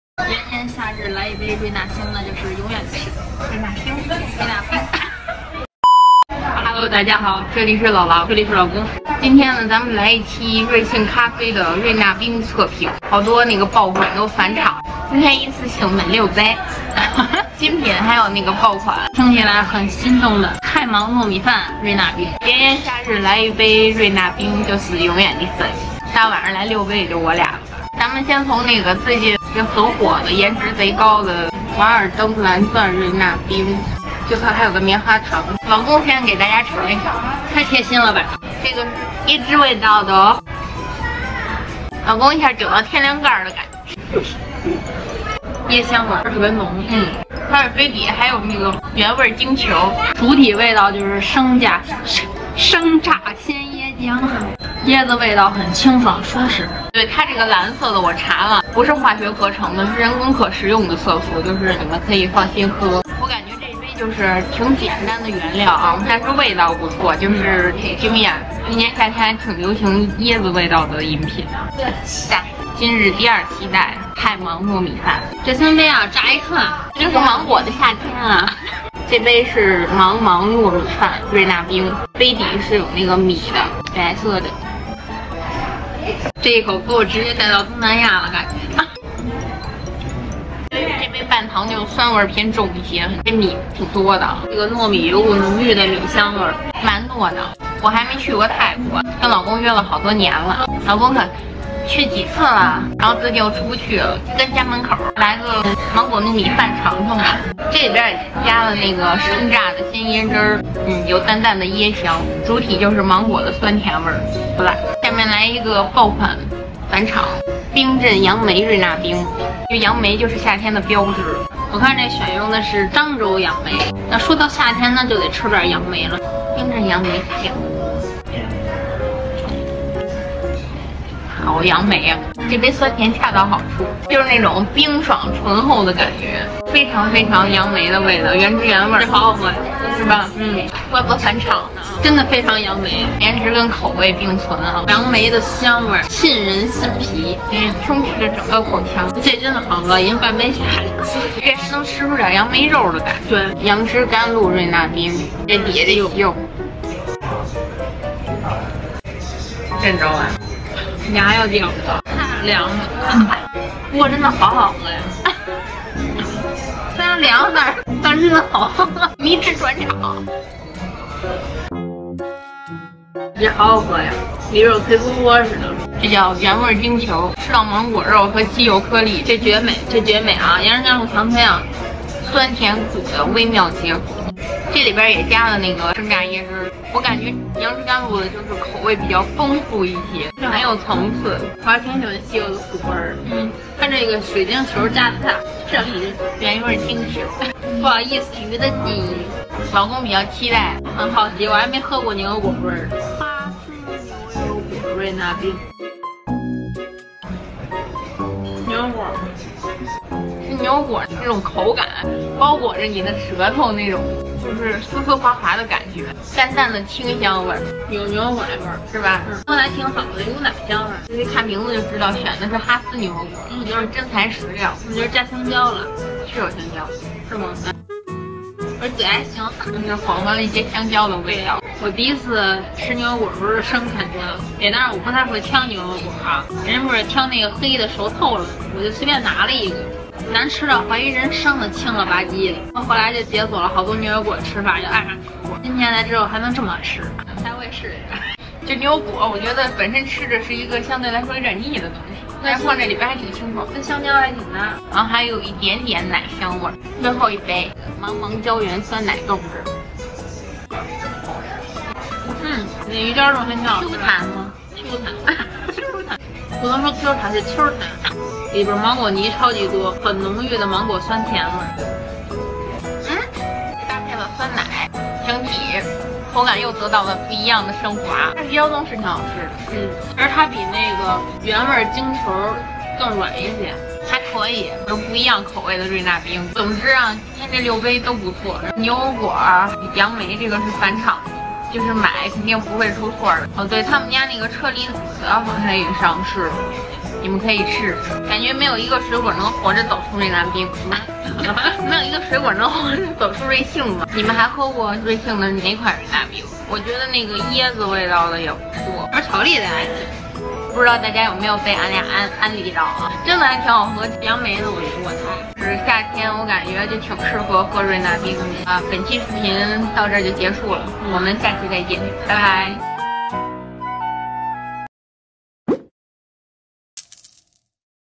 瑞纳冰大测评！蓝色沙冰？夏日必备 没有一个水果能活着走出瑞幸！
*瑞幸